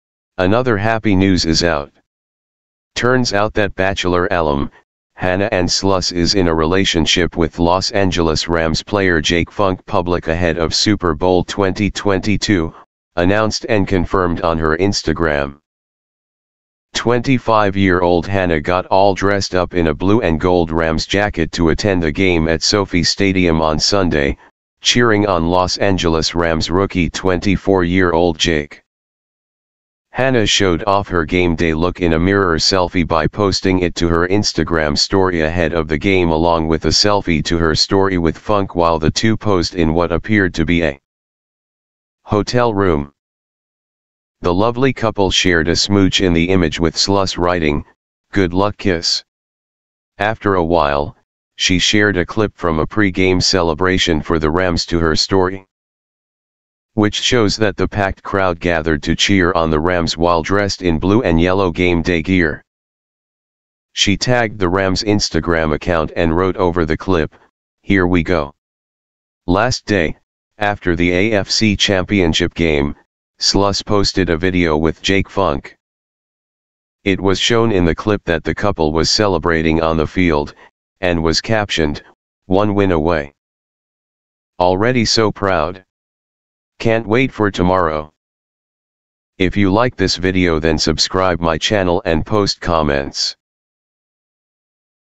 Rams' Jake Funk player number hints at how long he's dated Hannah Ann.
Hannah Ann Sluss is dating Los Angeles Rams running back Jake Funk. Jake is not a star player quite yet, but now he's dating Bachelor royalty.